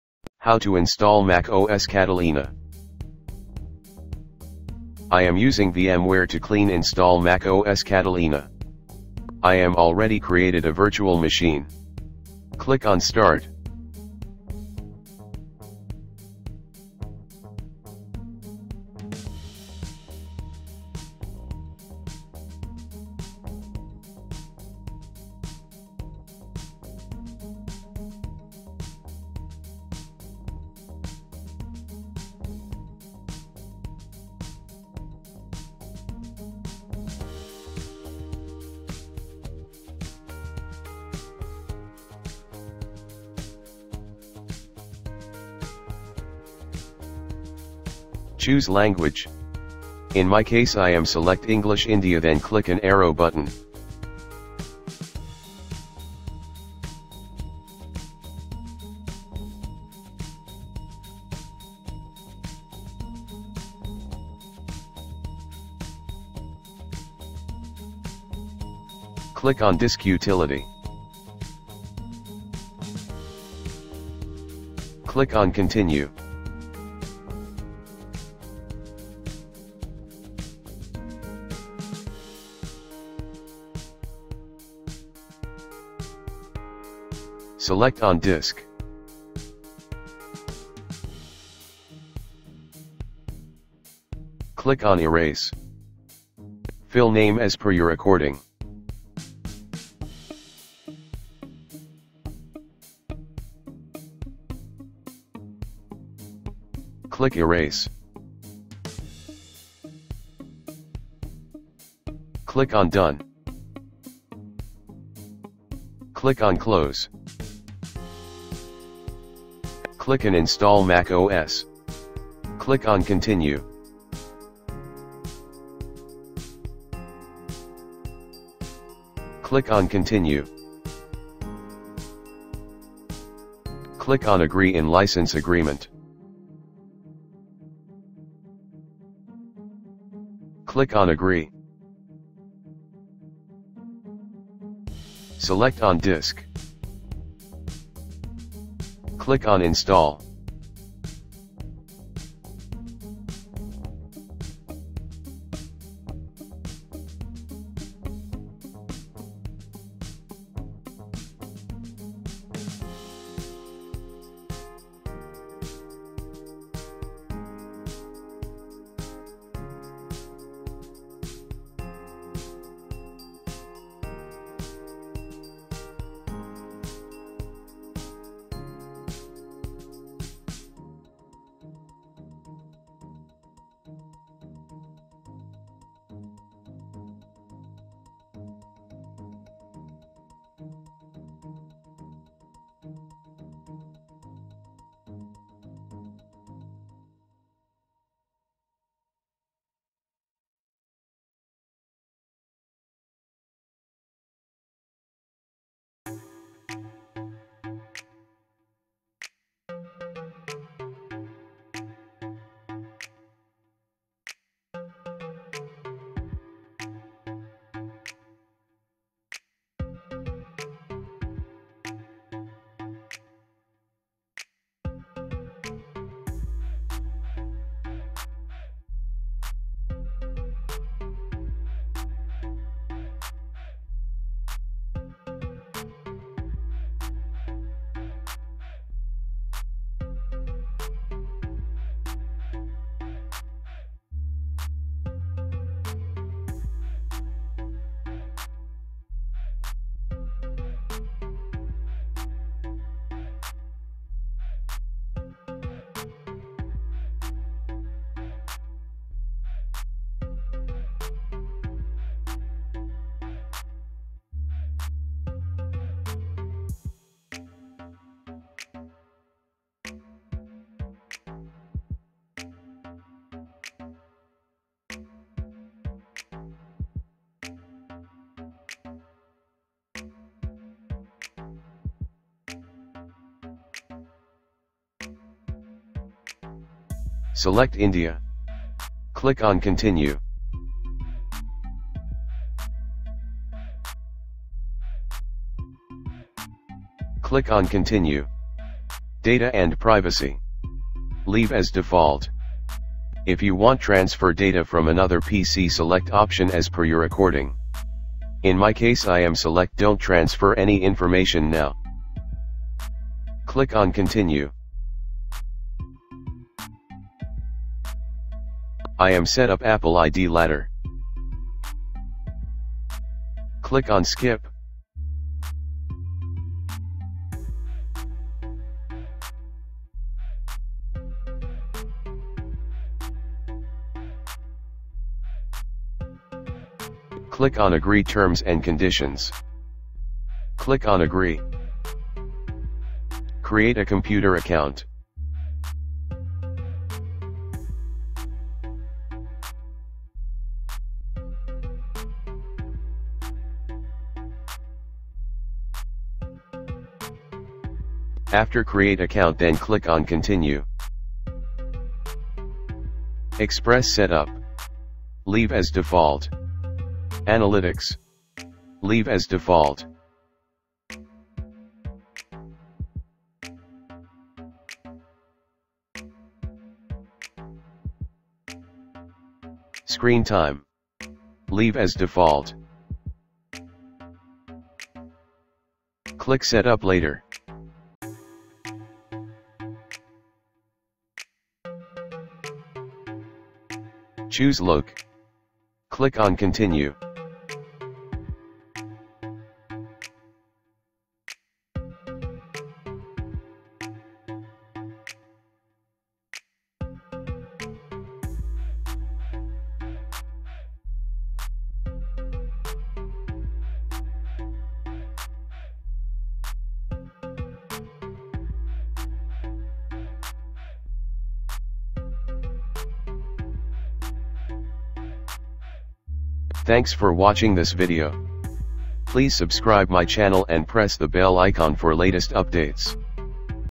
How to Clean Install macOs Catalina.
This Video is show on How to install macOs Catalina clean Install through DVD